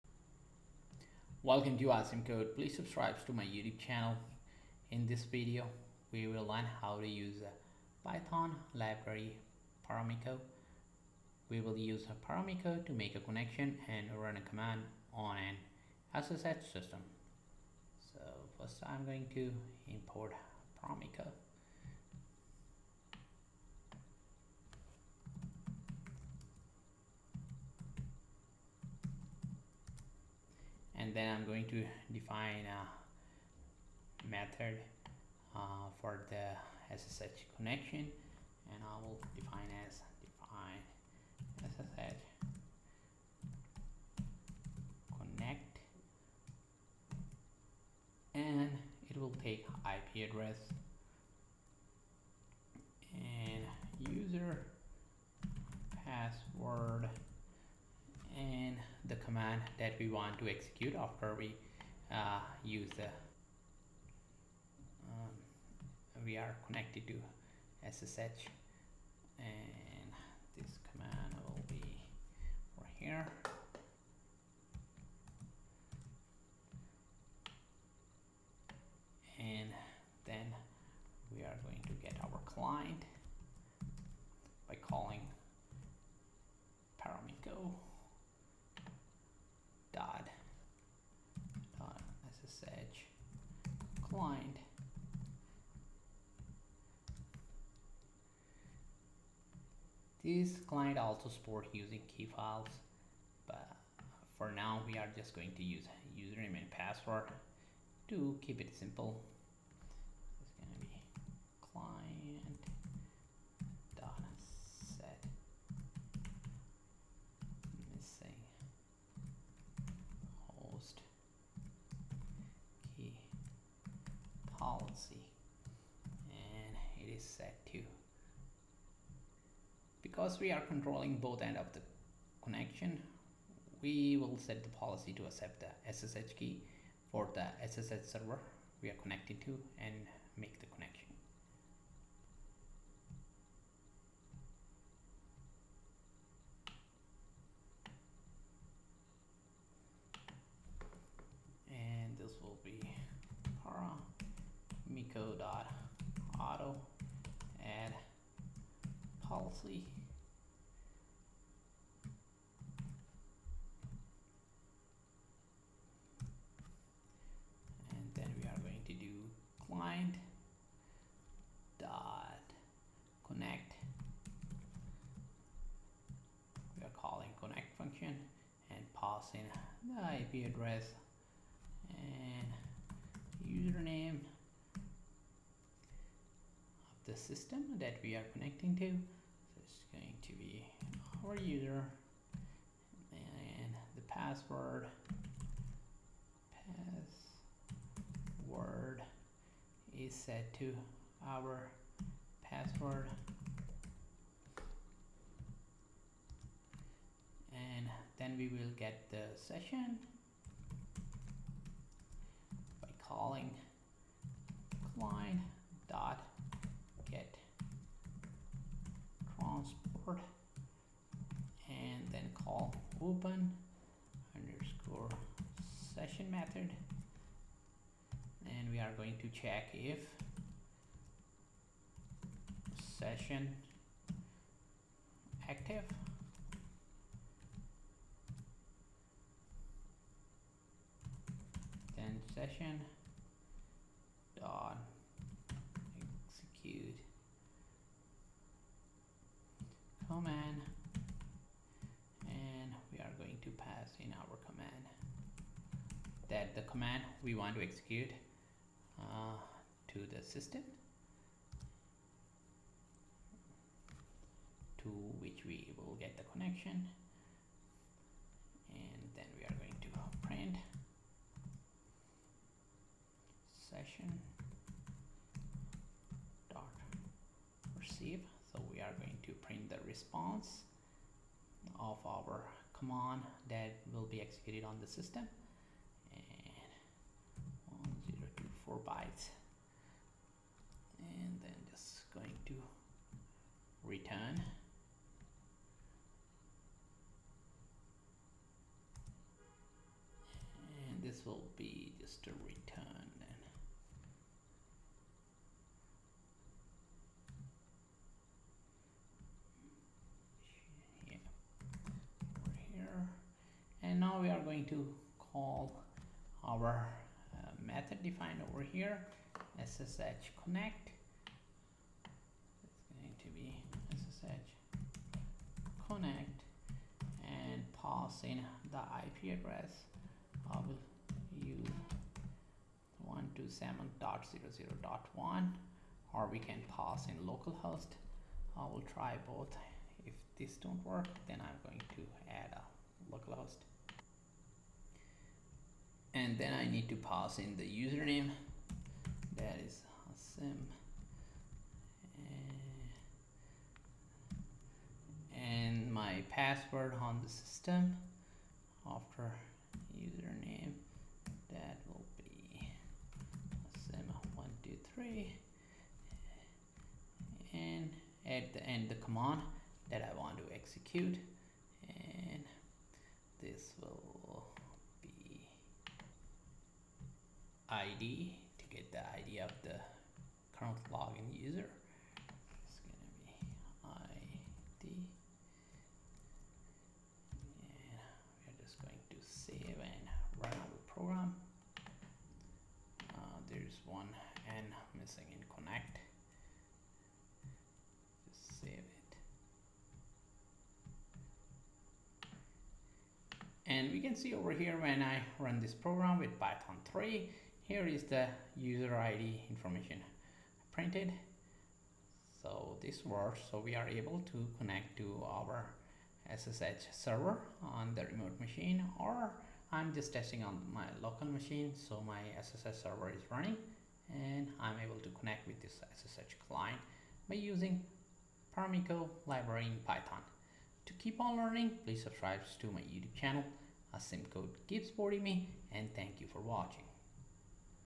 Python SSH with Paramiko
In this video we will learn how to use SSH with Paramiko library in Python.